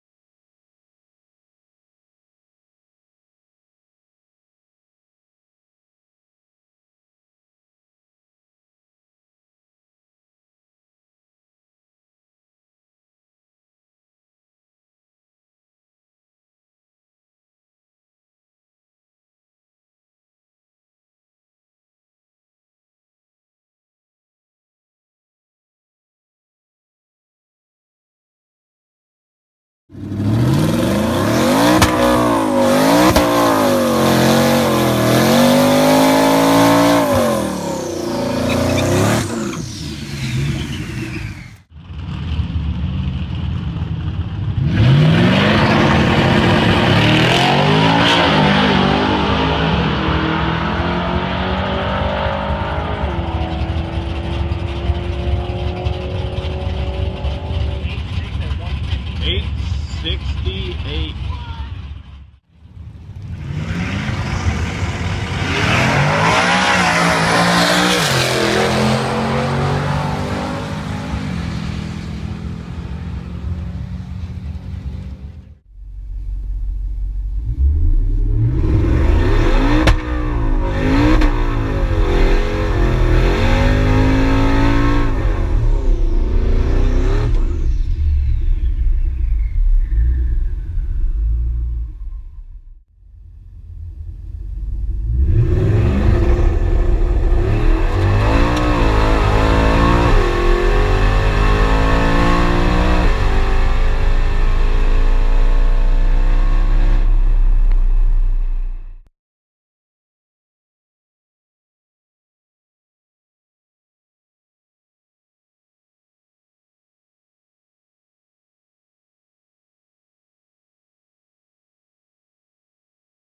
World's quickest and fastest stock motor 5.0 coyote mustang! 8.6@156!
JUIC3D's Snow WhiTTe
2014 Mustang GT
Hellion Twin Turbo kit
Precision 6266 turbos
BMR Suspension
Circle D torque converter
Self-tuned on E85 using HP Tuners
Fore fuel system (twin 465 pumps)
ID1000 injectors
~14psi

Bradenton Motorsports Park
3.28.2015